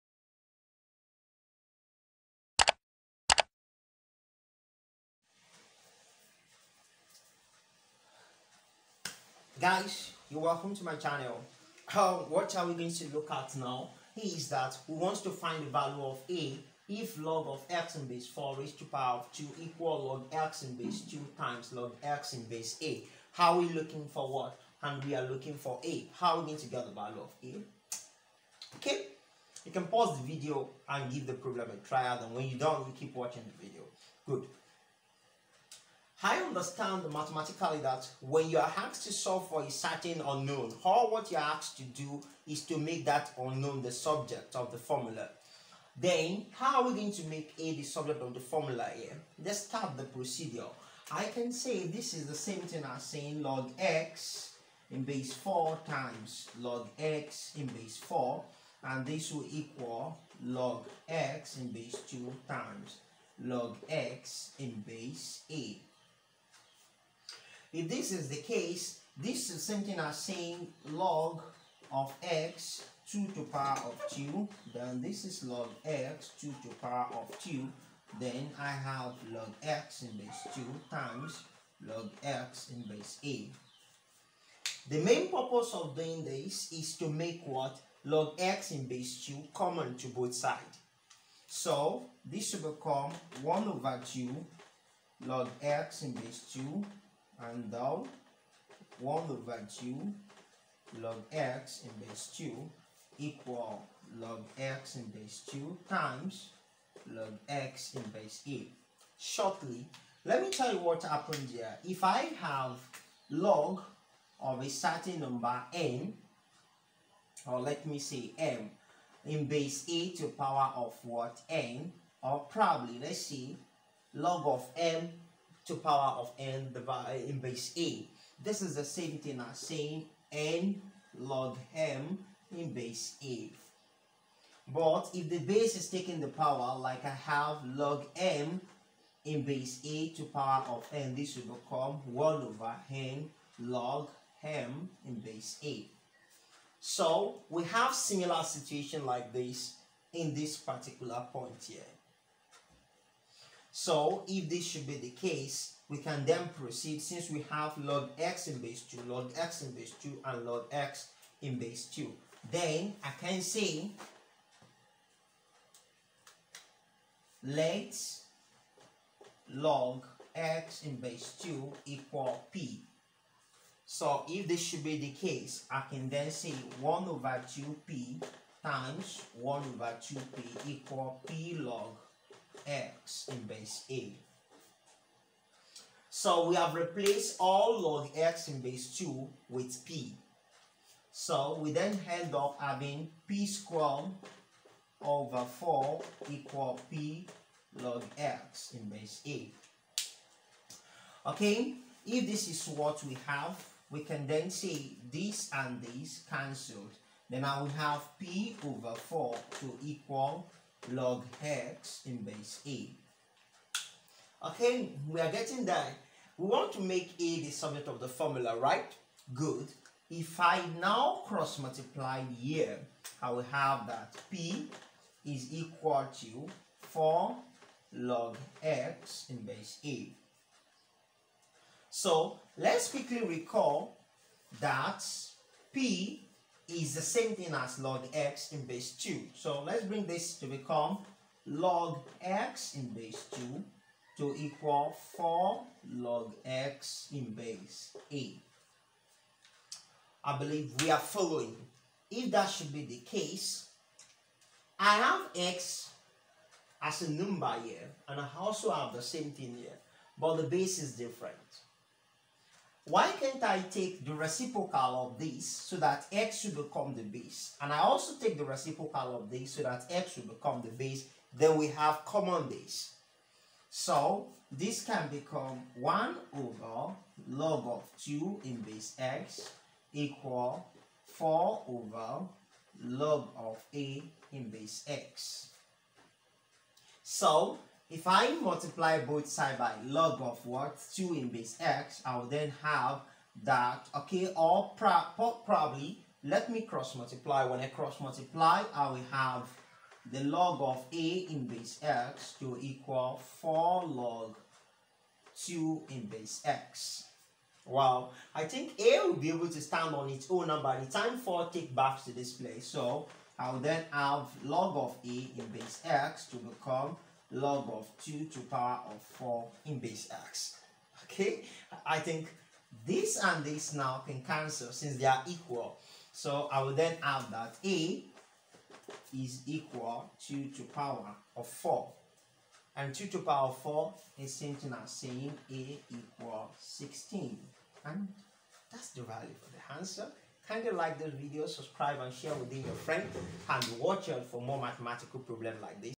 Logarithm with two unknowns
This video will help you on how to solve a logarithm problem with two unknowns without solving it simultaneously...